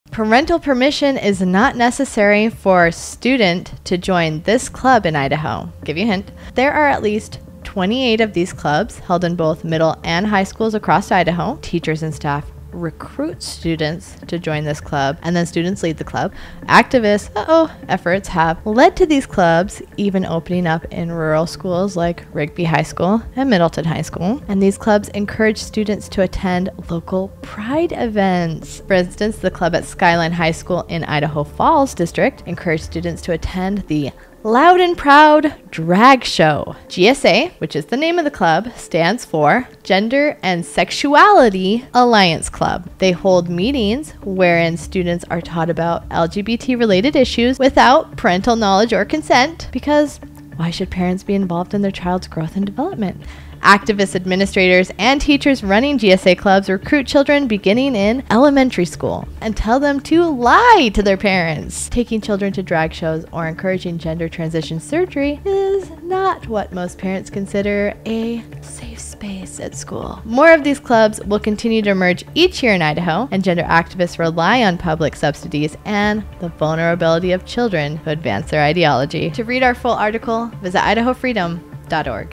Trans activists target Idaho students through school clubs
No parental permission required. In fact, lying to your parents is encouraged.

To read our full article, visit: idahofreedom.org